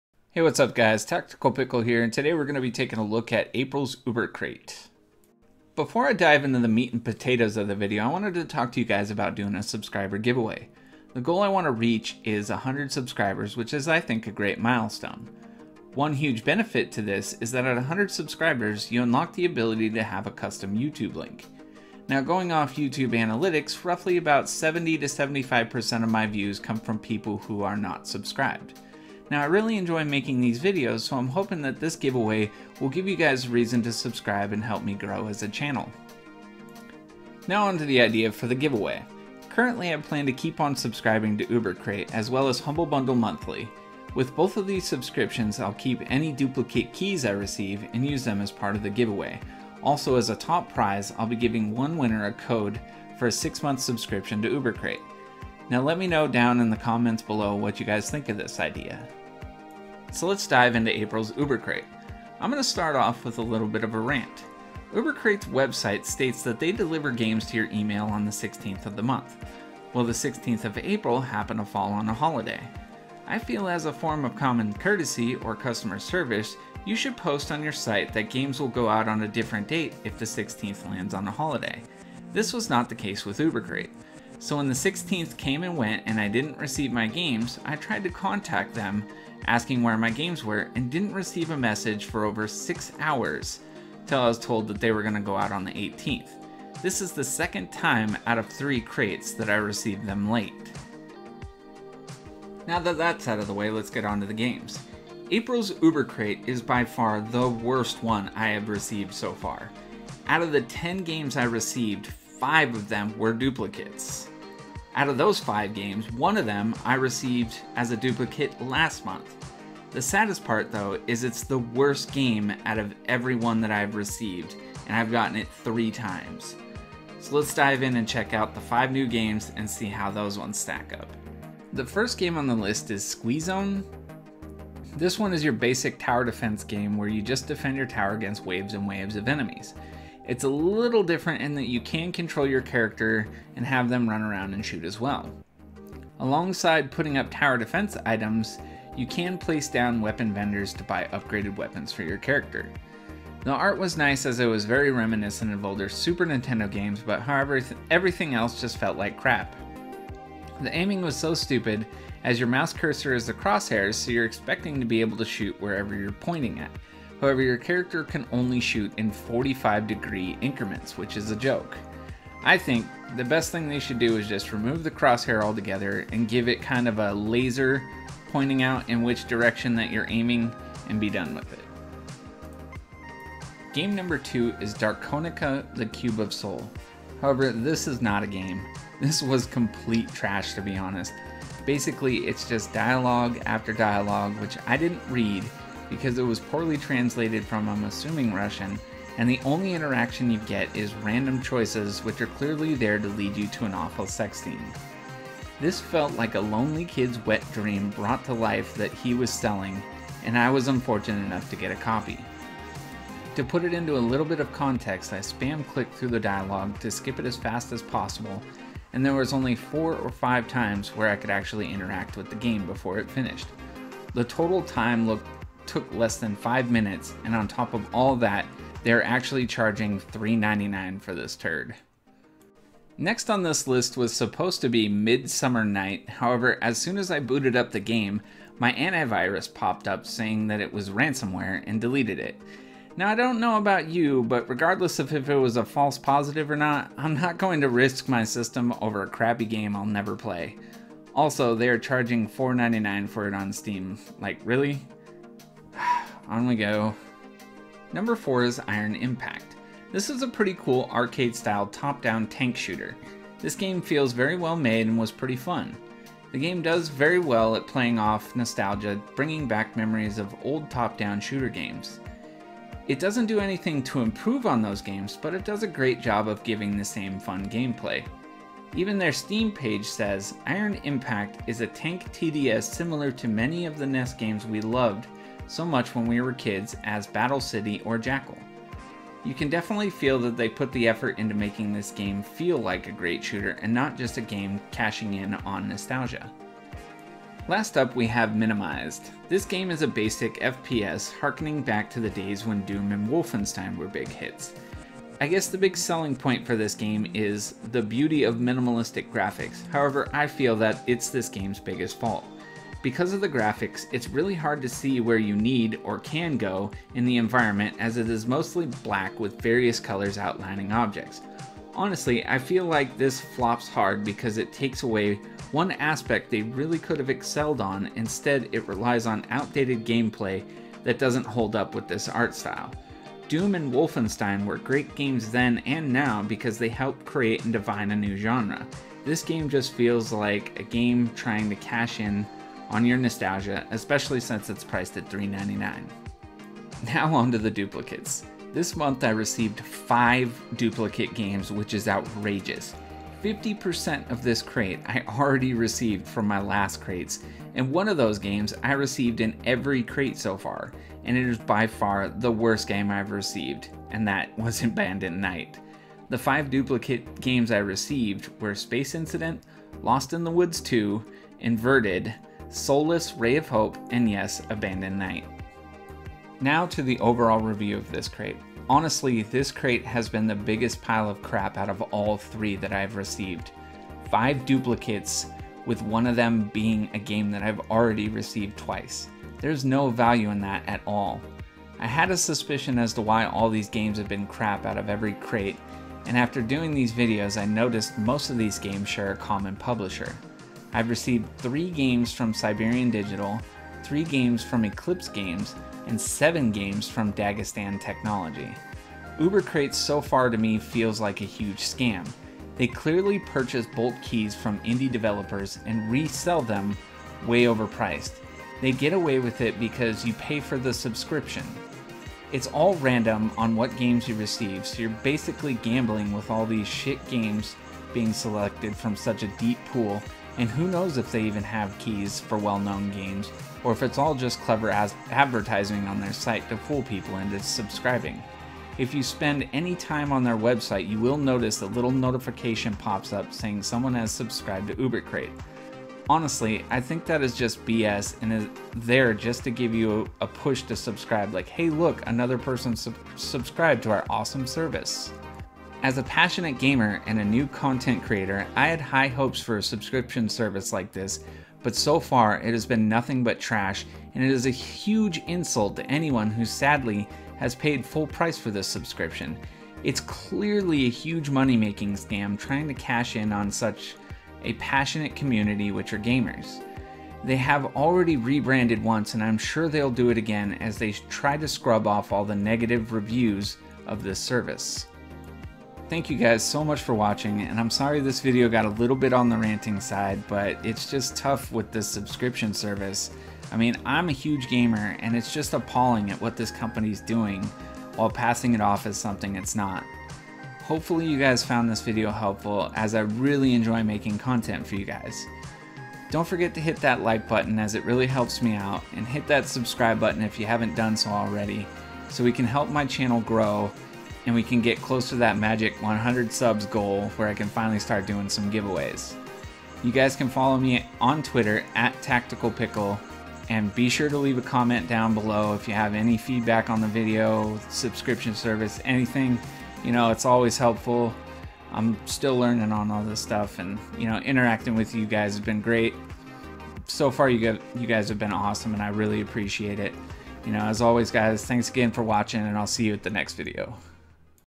Ubercrate Review April 2017
Thank you guys for all the awesome support! Todays video I go over my april ubercrate as well as talk about my up coming subscriber giveaway!